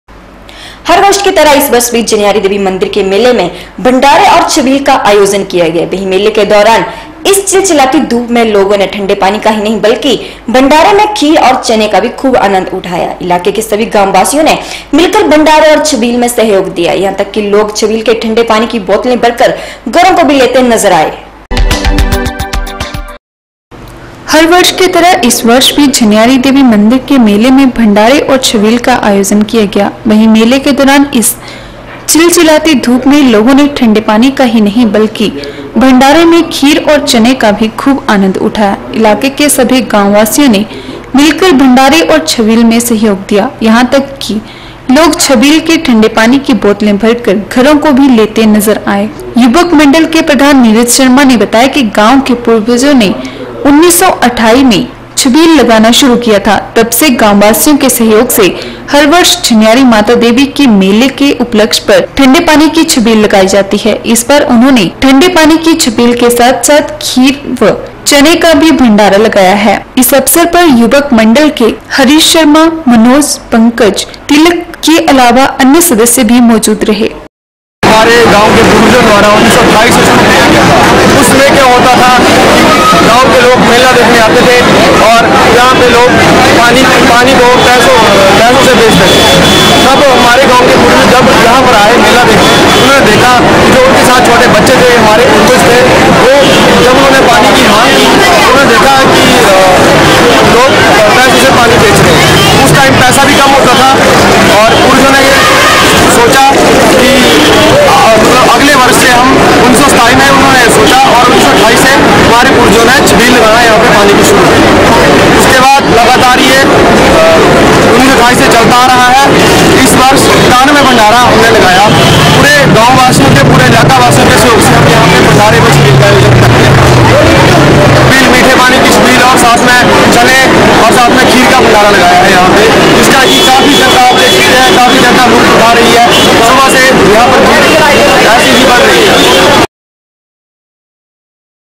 11 JUNE N 5 This year also organized Chhevil in the Jhanari Devi Temple Fair
हर वर्ष की तरह इस वर्ष भी झनियारी देवी मंदिर के मेले में भडारे और छवील का आयोजन किया गया। वहीं मेले के दौरान इस चिलचिलाती धूप में लोगों ने ठंडे पानी का ही नही ब्लकि भंडारेमें खीर और चने का भी खुब आनंद उठाया ।